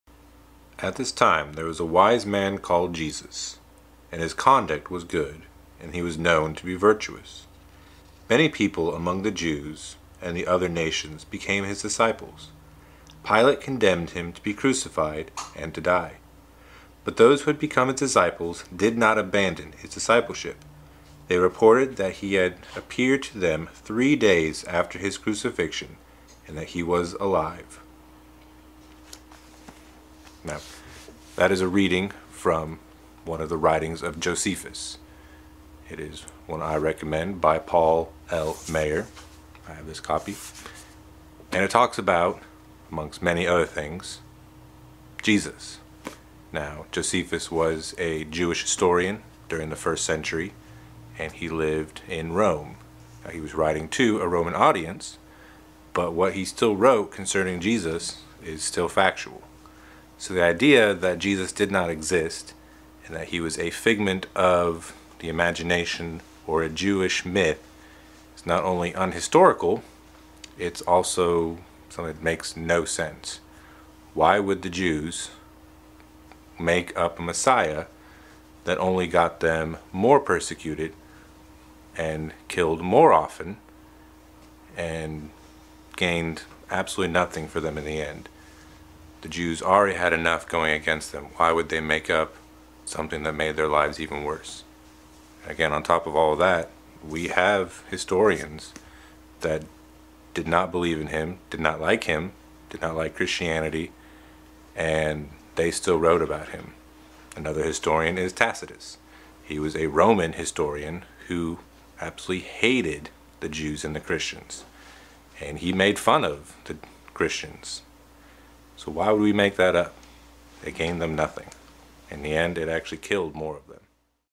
Reading From Josephus
Josephus was a first century jewish historian who mentions jesus in his book Jewish Antiquities. This is an important document used by secular and christian historians alike.